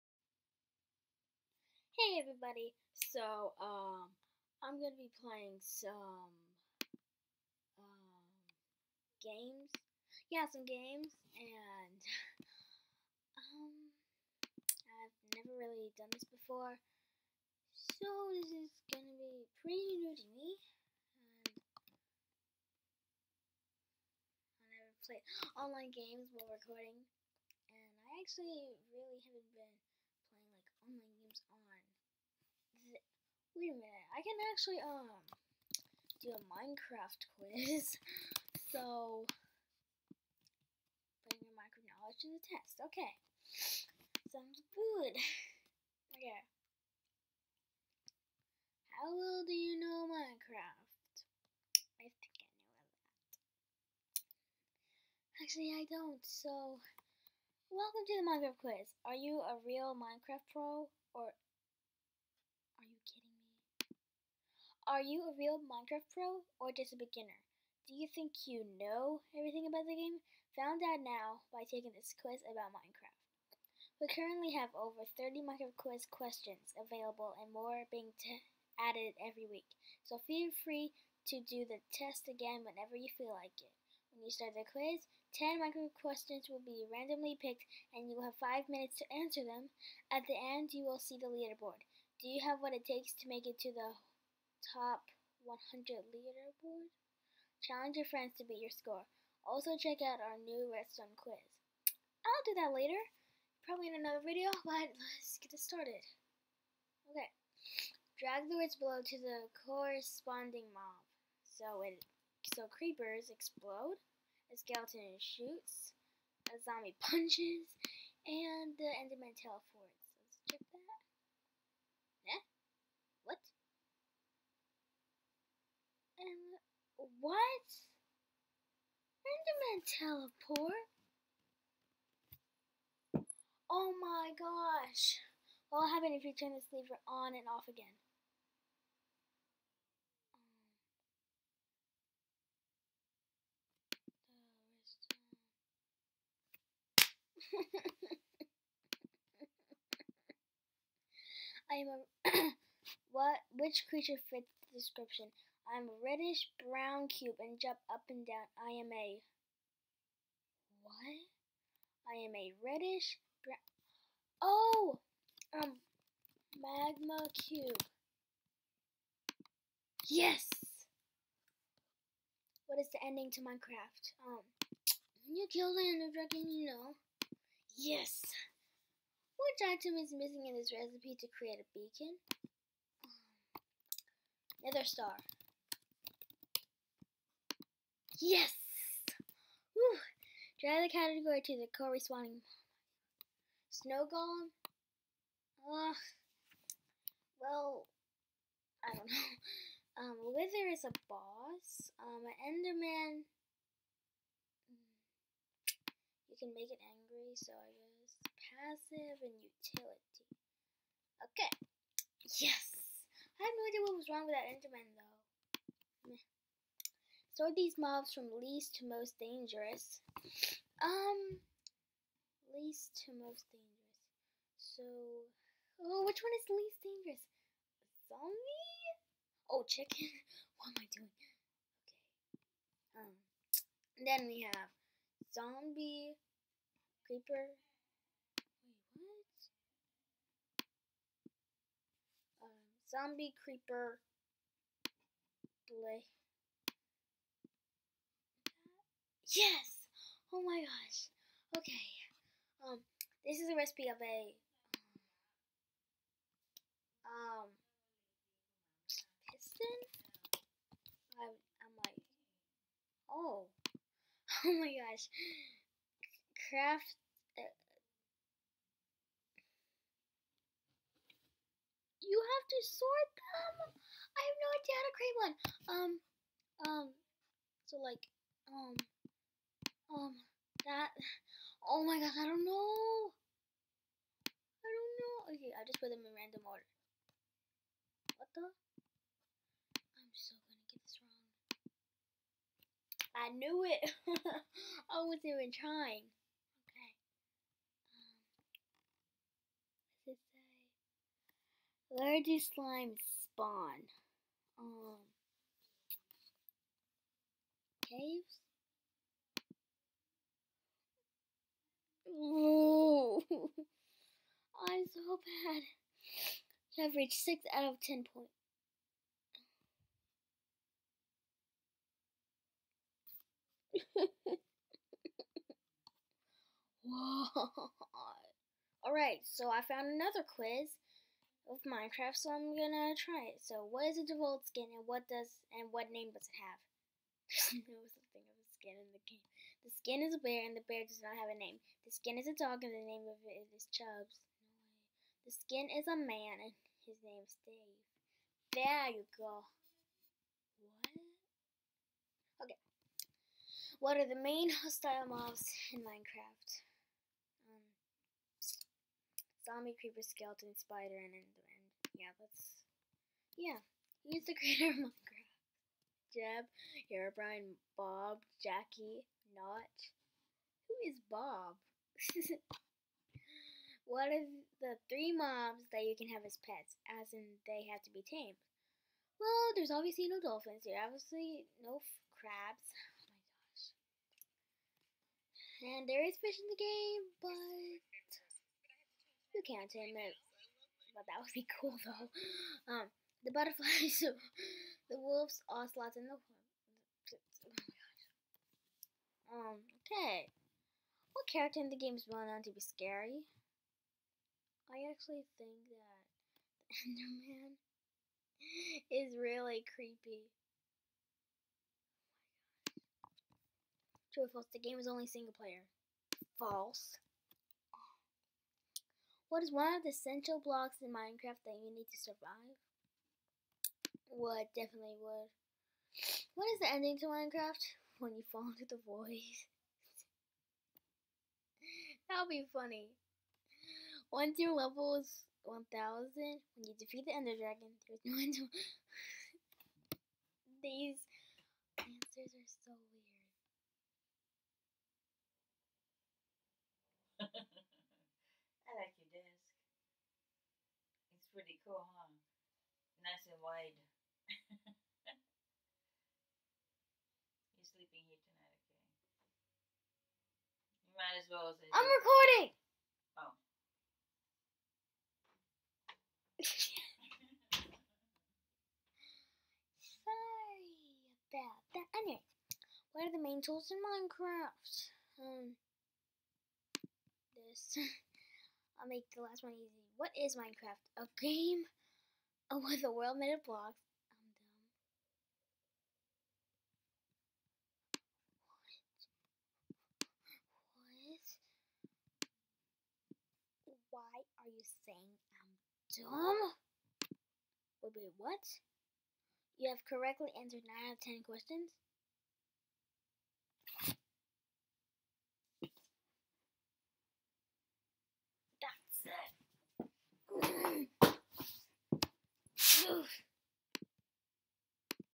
Website Fun! | MINECRAFT QUIZ!
I decided to try some minecraft quizzes... I really need to learn more about Minecraft Lol!

- Chloe